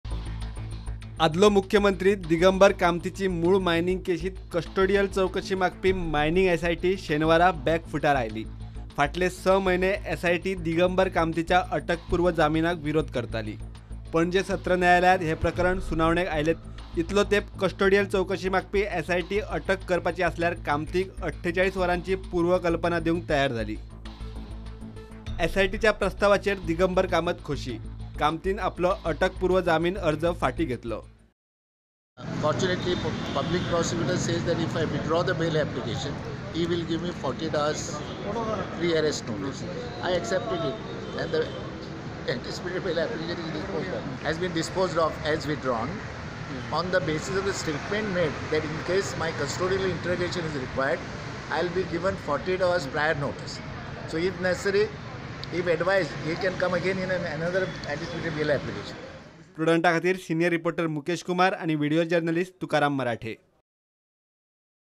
SIT (MINES) COMPROMISES WITH DIGAMBAR KAMAT _Prudent Media Goa
Panaji Sessions Court disposes off Digambar Kamat's anticipatory bail application as 'Withdrawn' in main mining cases